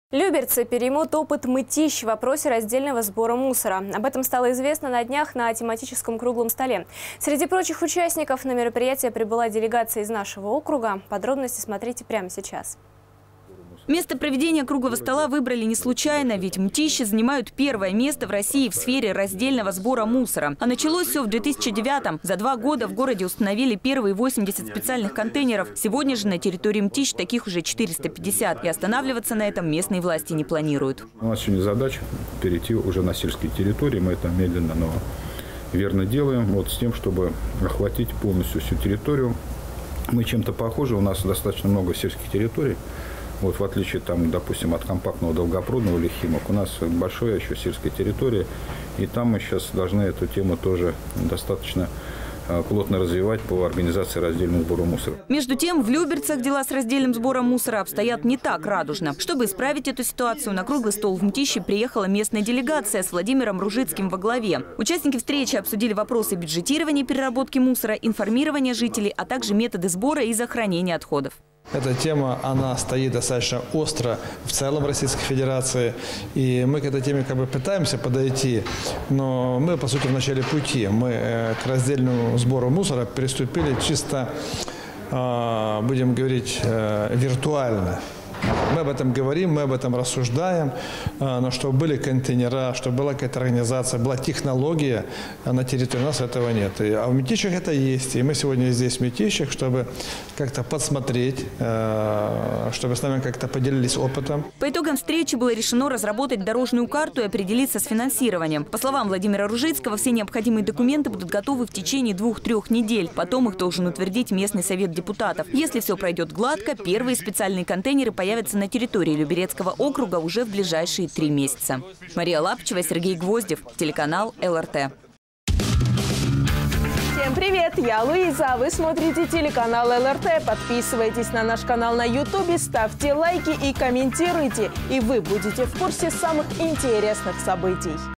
Мытищи поделились опытом по раздельному сбору мусора
Люберцы переймут опыт Мытищ в вопросе раздельного сбора мусора. Об этом стало известно на днях, на тематическом круглом столе. Среди прочих участников, на мероприятие прибылА и делегация из нашего округа. Подробности смотрите прямо сейчас.

Место проведения круглого стола выбрали не случайно, ведь Мытищи занимают первое место в России в сфере раздельного сбора мусора. А началось все в 2009. За два года на в городе установили первые 80 специальных контейнеров. Сегодня же на территории Мытищ таких уже 450. И останавливаться на этом местные власти не планируют.

Виктор Азаров, глава г. о. Мытищи:
«У нас сегодня задача перейти на сельские территории. Мы это медленно, но верно делаем. С тем, чтобы охватить полностью всю территорию. Мы чем-то похожи. У нас достаточно много сельских территорий, в отличие от там компактного Долгопрудного или Химок. У нас достаточно большая еще сельская территория. И там мы должны эту тему достаточно плотно развивать по организации раздельного сбора мусора».

Между тем, в Люберцах дела с раздельным сбором мусора обстоят не так радужно. Чтобы исправить эту ситуацию, на круглый стол в Мытищи приехала местная делегация с Владимиром Ружицким во главе. Участники встречи обсудили вопросы бюджетирования переработки мусора, информирования жителей, а также методы сбора и захоронения отходов.

Владимир Ружицкий, глава г. о. Люберцы:
"Эта тема стоит достаточно остро в целом в Российской Федерации. И мы к этой теме пытаемся подойти, но мы, по сути, в начале пути. Мы к раздельному сбору мусора приступили чисто виртуально. Мы об этом говорим, мы об этом рассуждаем, чтобы были контейнера, чтобы была какая-то организация, чтобы была технология на территории. У нас этого нет. А в Мытищах это есть. И мы сегодня здесь, в Мытищах, чтобы как-то посмотреть, чтобы с нами поделились опытом».

По итогам встречи было решено разработать «дорожную карту» и определиться с финансированием. По словам Владимира Ружицкого, все необходимые документы будут готовы в течение 2-3 недель, потом их должен утвердить местный совет депутатов. Если все пройдет гладко, первые специальные контейнеры появятся на территории люберецкого округа уже в ближайшие 3 месяца.

Мария Лапычева, Сергей Гвоздев